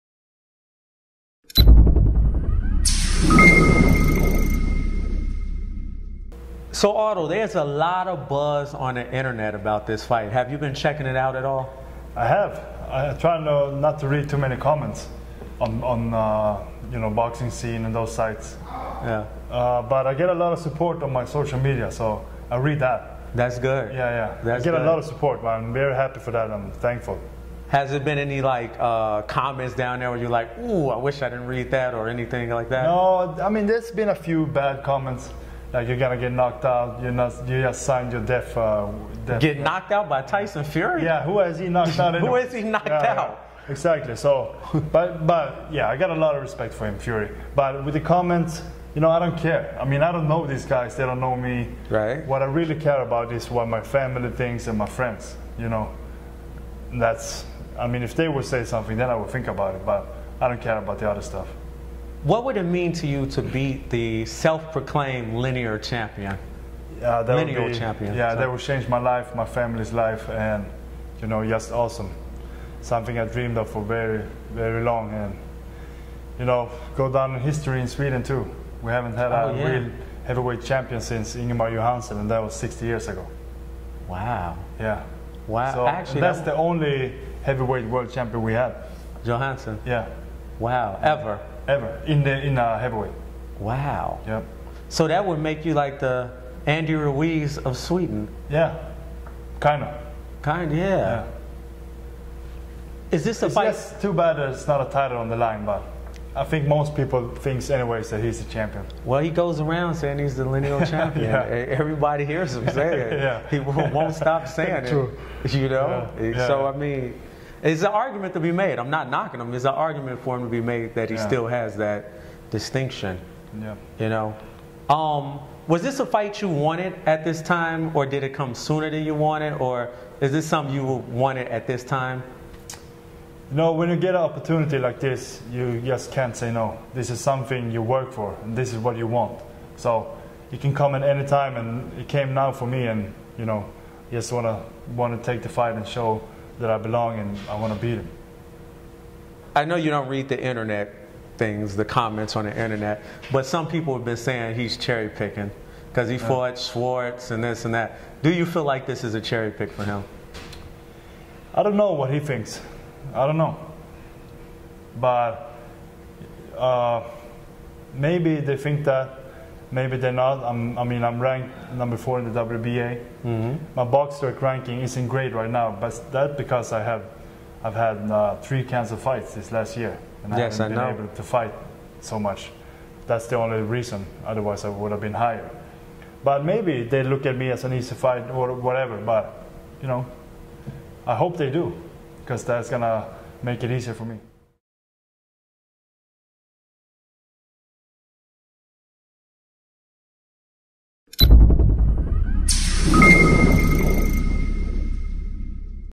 DEFEATING TYSON FURY MAKES ME THE ANDY RUIZ OF SWEDEN--OTTO WALLIN
Otto Wallin discusses his upcoming fight against Tyson Fury